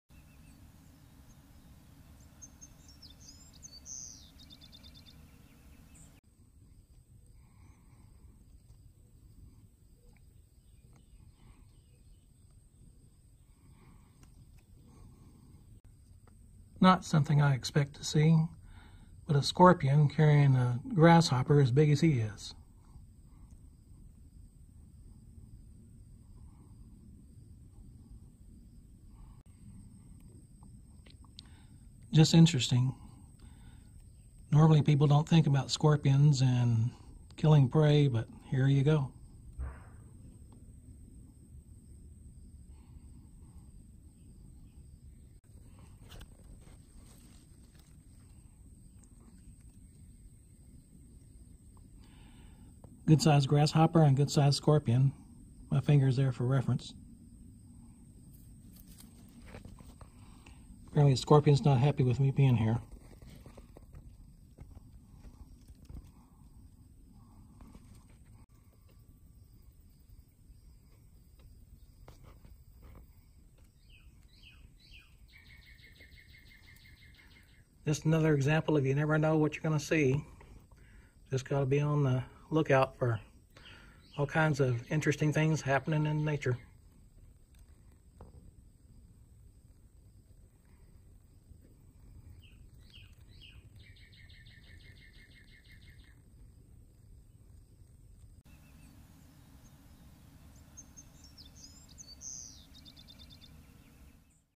Texas Trail Cam - Scorpion 1 - Grasshopper 0... July 2019
Just happened upon a bark scorpion dragging  large grasshopper it had captured near the barn this morning.  Too good to pass up for a quick video of an awesome predator with its prey even if they are both arthropods.  Nature just showing her stuff & how things work.